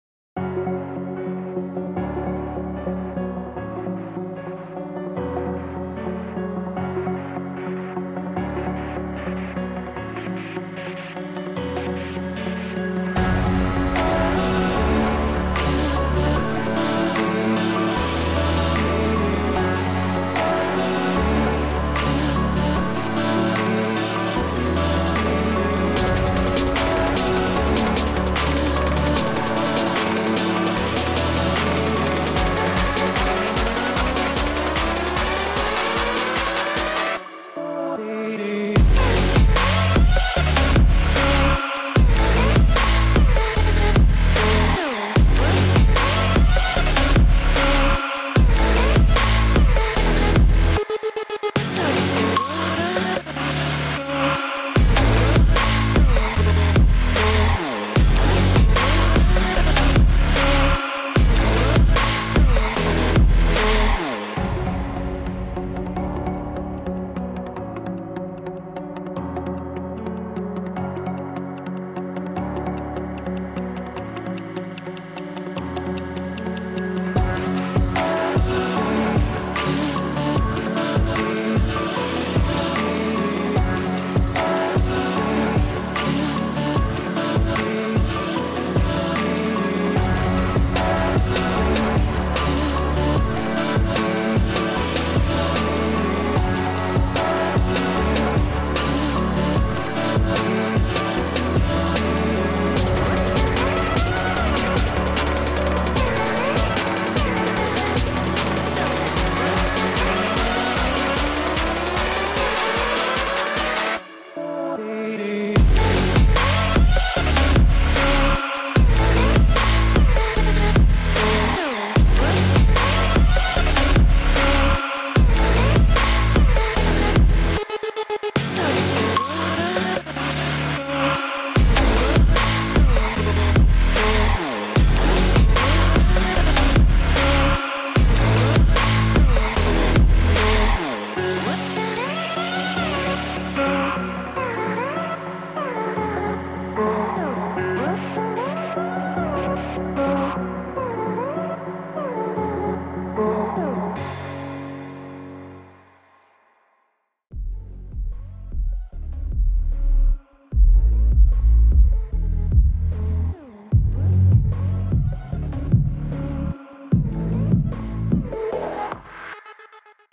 16 XIAOMI ТОВАРОВ с ALIEXPRESS за 2.5 минуты!
Парад товаров Xiaomi с Aliexpress в честь завершения праздничной распродажи